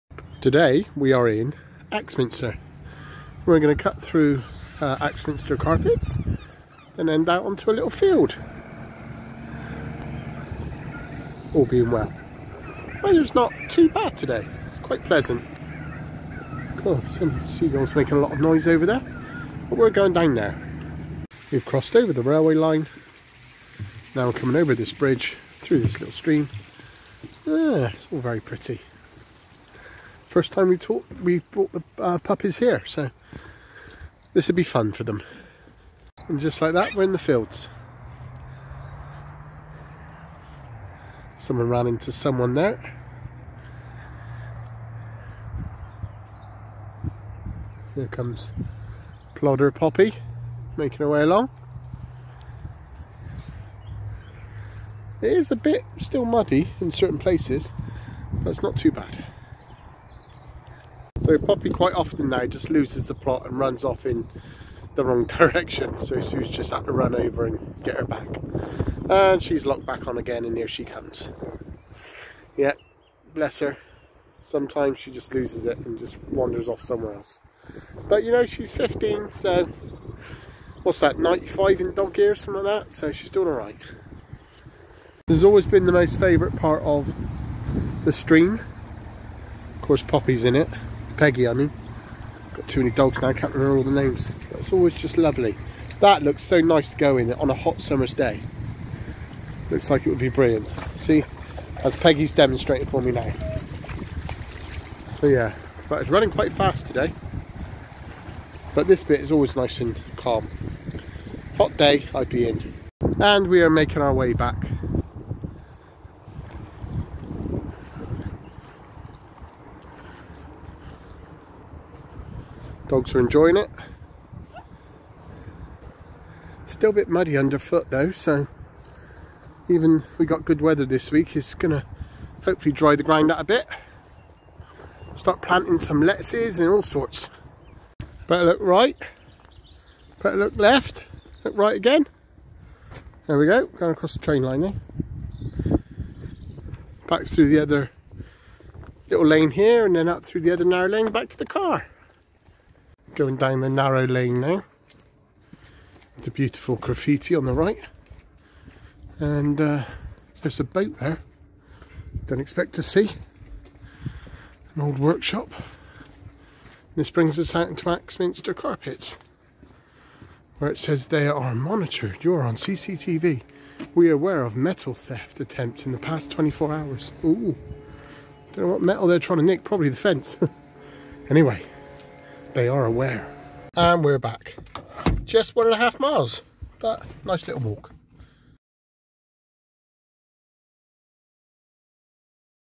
A short 1.5 Mile walk in Axminster
A short 1.5 Mile walk in Axminster . Crossing trainline and through fields